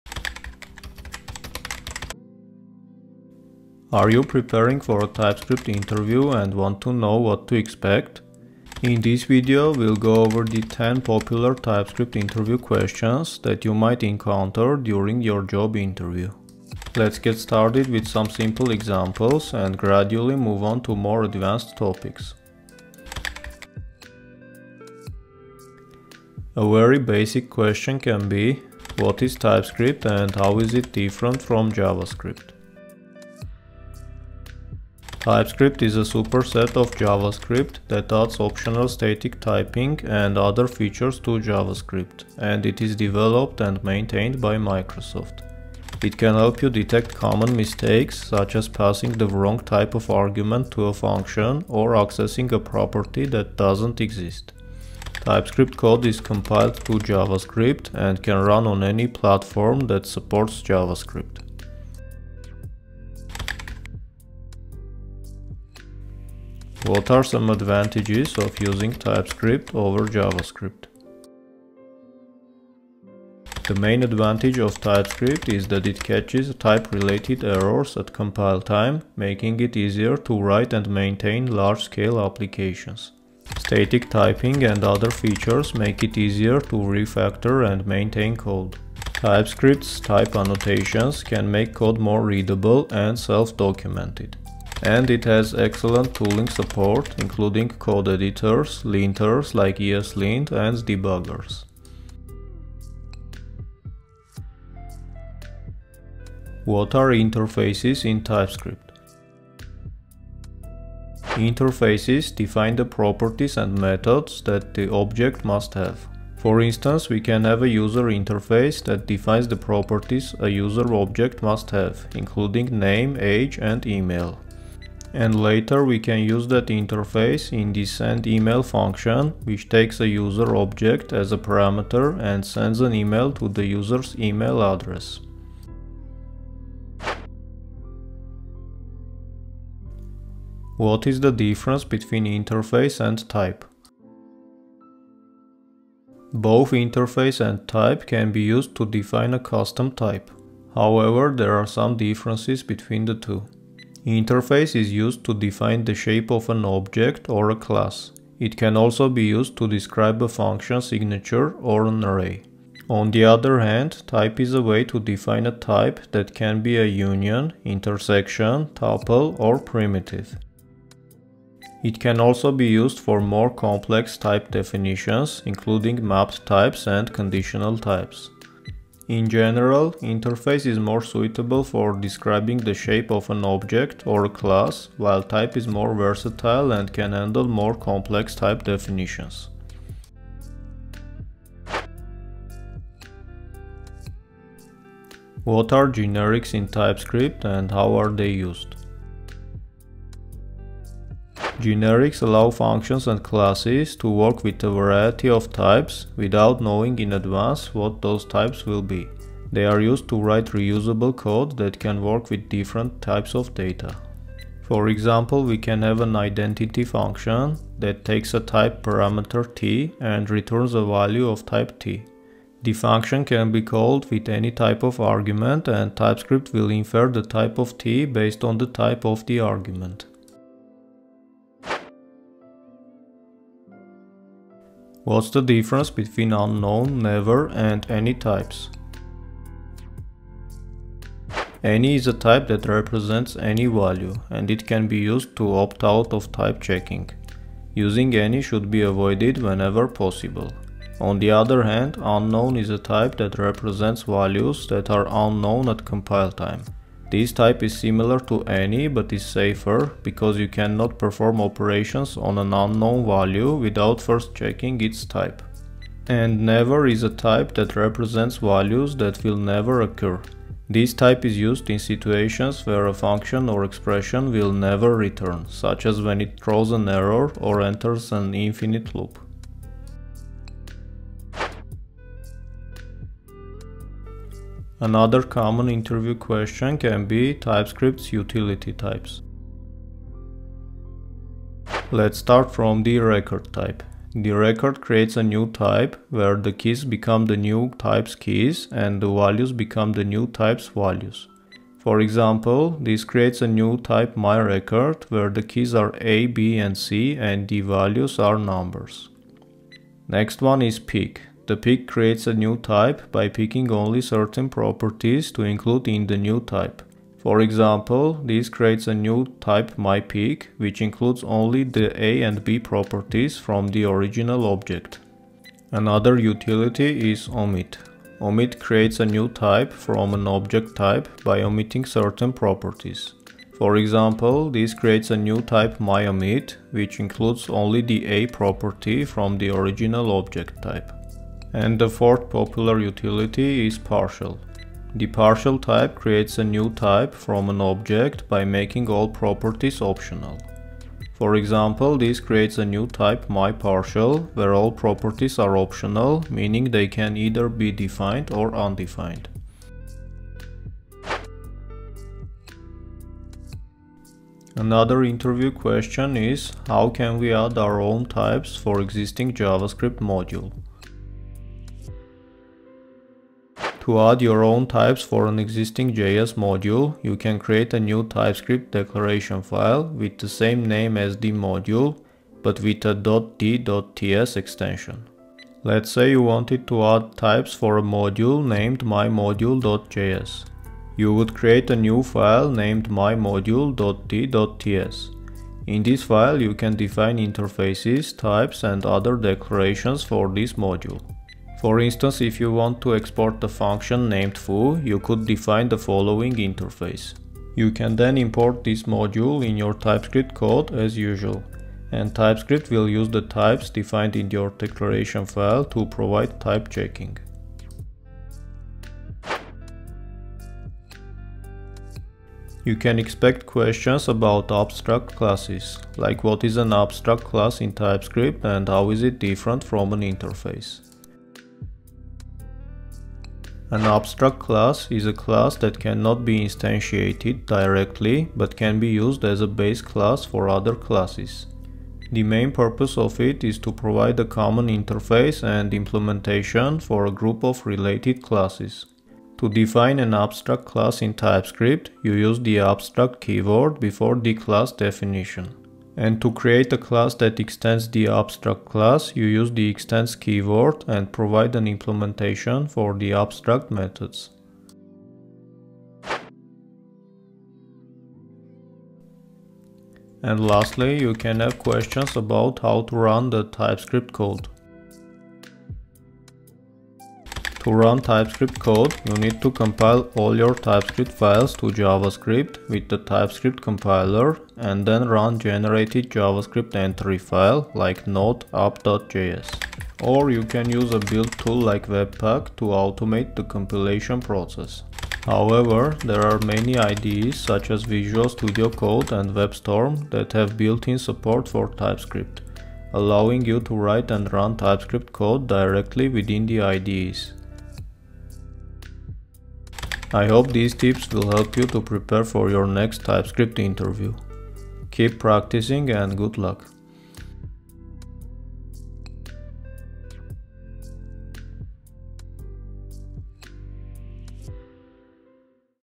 Master TypeScript Interviews
Are you preparing for a TypeScript interview and want to know what to expect? Check out the frequently asked questions during the interview.

🔖 Sections 🔖
0:00 Introduction
0:22 Question 1
1:04 Question 2
1:45 Question 3
2:18 Question 4
3:13 Question 5
3:54 Question 6
4:41 Question 7
6:00 Question 8
7:00 Question 9
7:47 Question 10